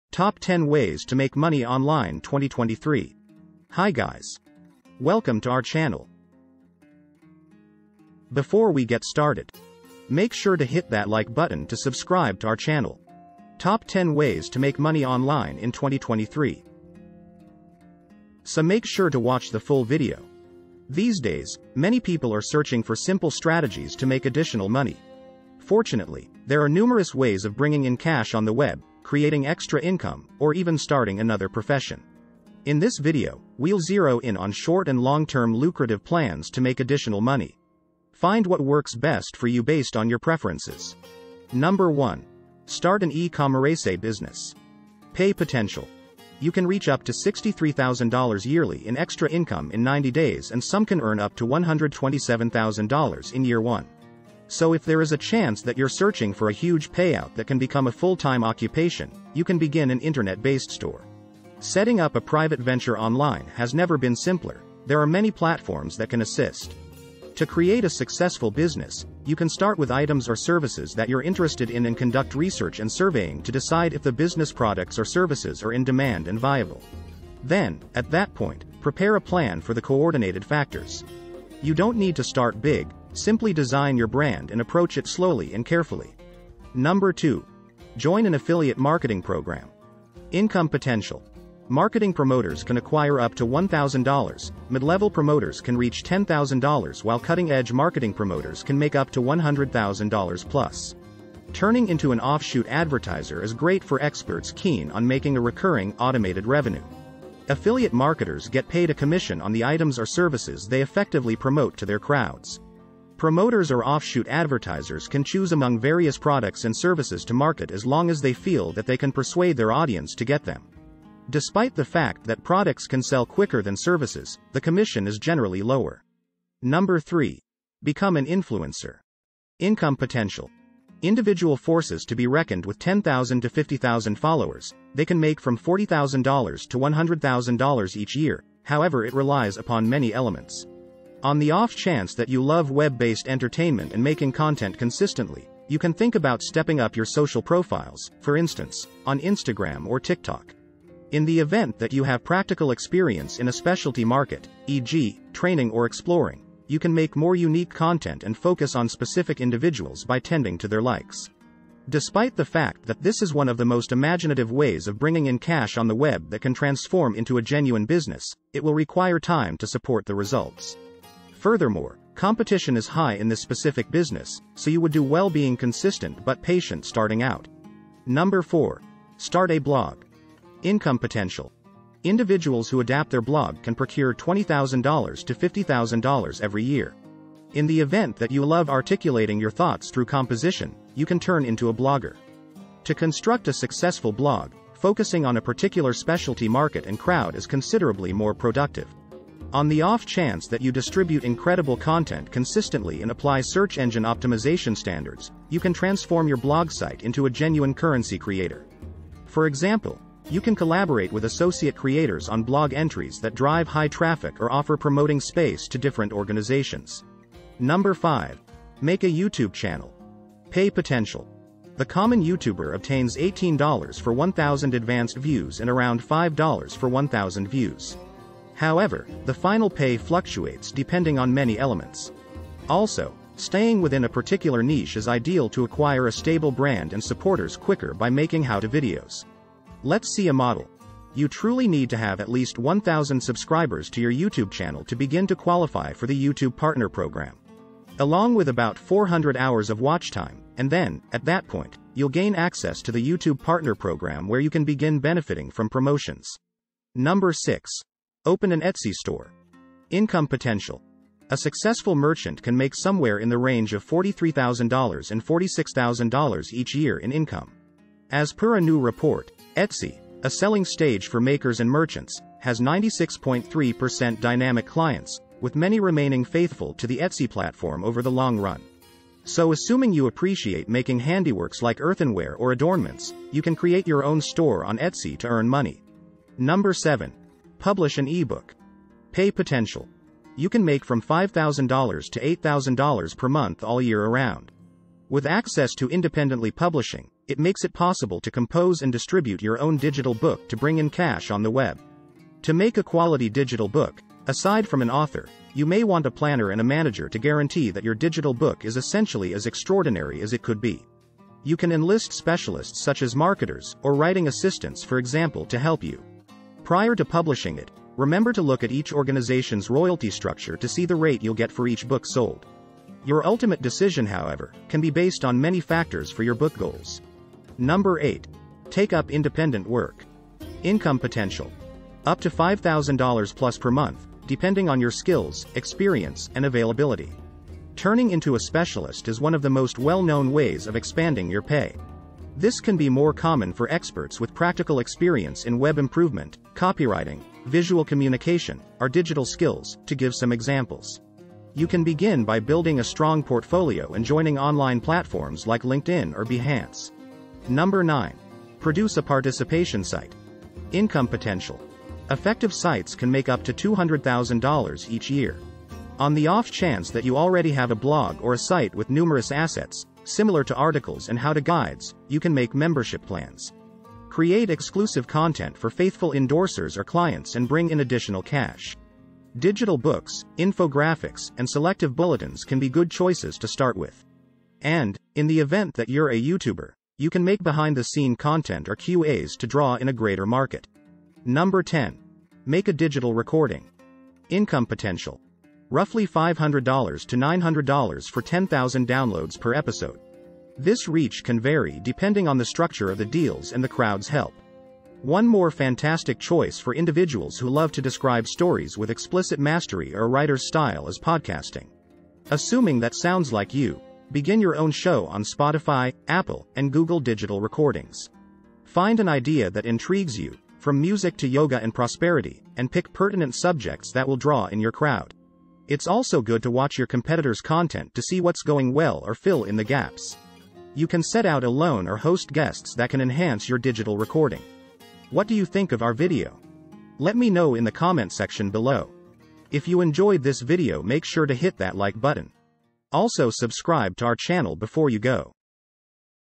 10 Ways To Make Money Online Must Watch And Learn / Earn Money Online
Todays video we are going to Discover 10 Different Ways To Make Money Online Must Watch And Learn / Earn Money Online

I hope this video helps you and inspires you
We would like to hear  if you have any comments

See You On our Next Inspiring Upload

0:00 Introduction
0:42 Start an E-commerece Business
2:07 Become an Influencer
2:59 Start A Blog
3:38 Make A YouTube Channel
4:47 Publish an E-book
5:28 Take Up Independent Work
5:58 Produce A Participation Site